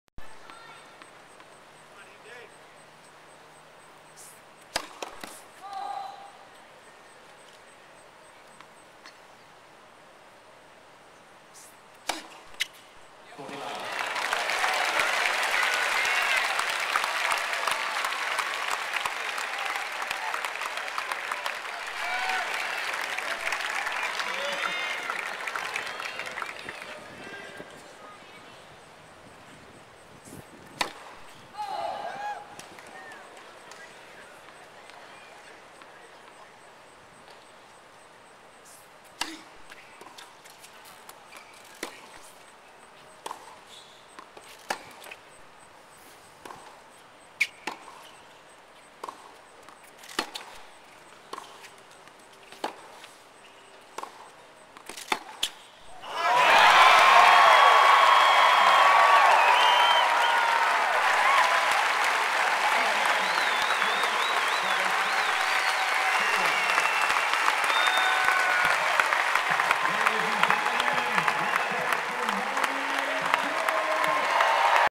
Mardy Fish Final Match Point in win over Andy Roddick
Mardy Fish Winning the Final Match Point in win over Andy Roddick at the Western & Masters Open